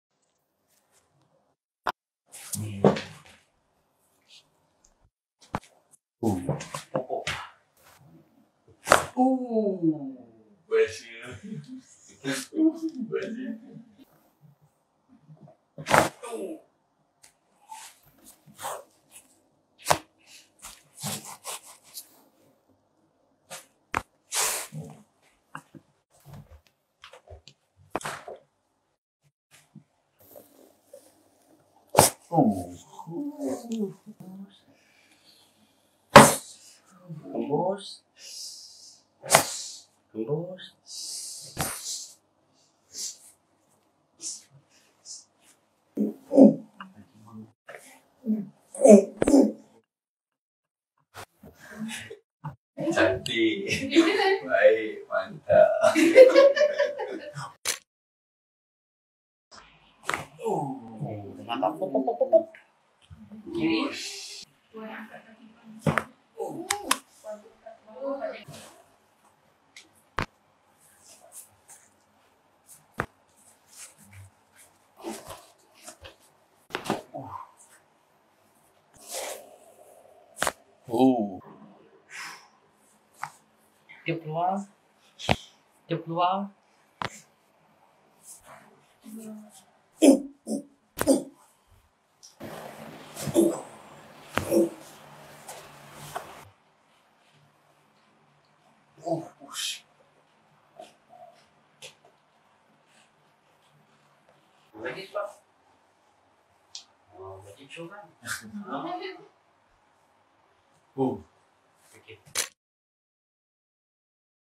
Celebrity Harris Annuar Knee Pain Gone Instantly⚠️
Harris Annuar (Malaysian Celebrity) menghadapi sakit lutut selama 2 tahun selepas surgeri. Sakit lutut hilang, badan lesu hilang selepas rawatan.